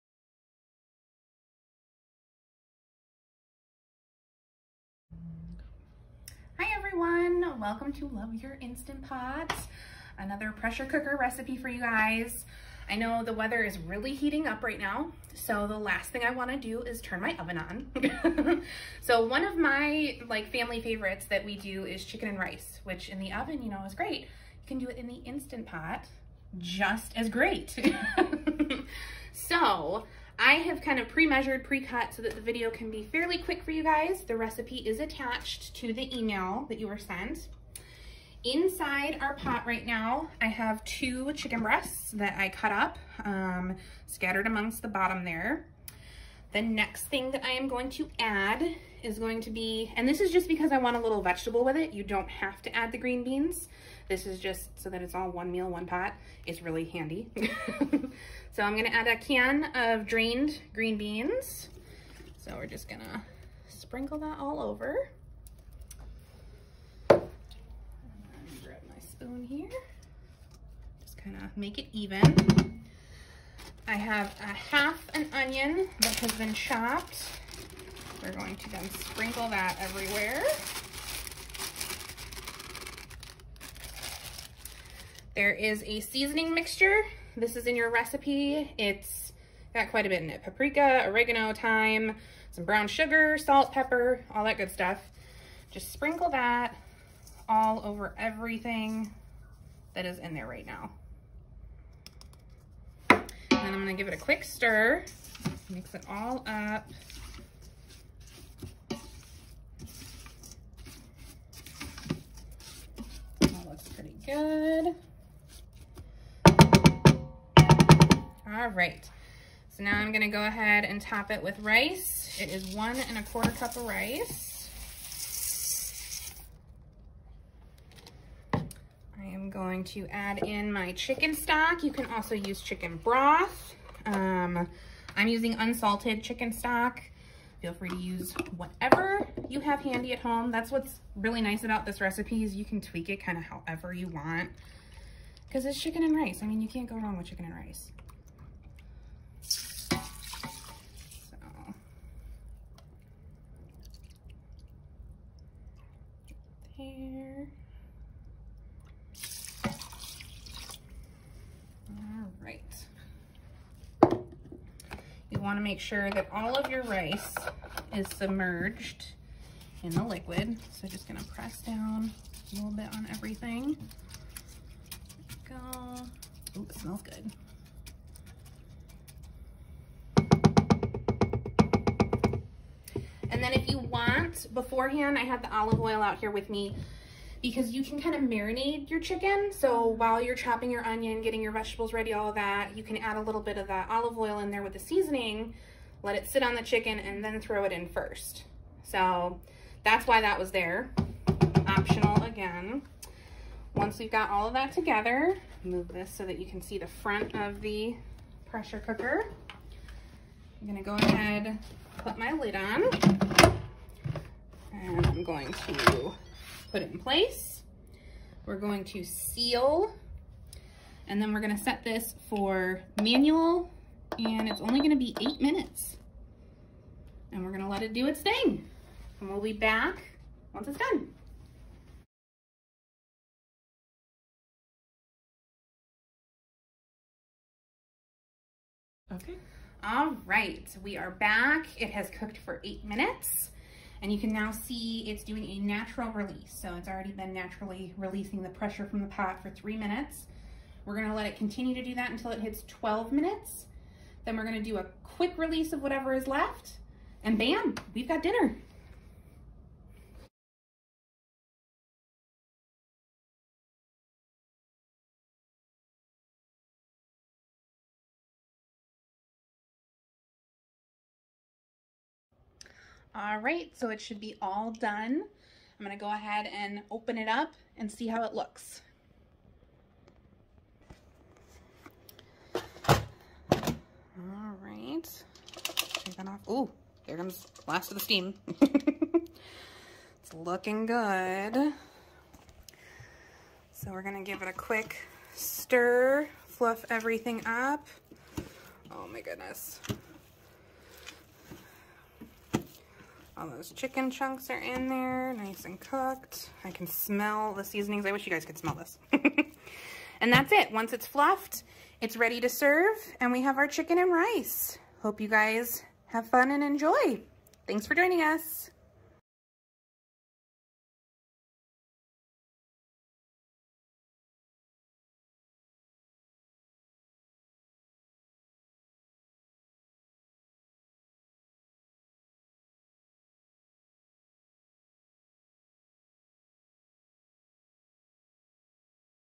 Shorewood-Troy Public Library - Love Your Instant Pot - Chicken & Rice
Shorewood-Troy Public Library staff show patrons how to use their pressure cookers to make a family favorite oven meal, chicken and rice!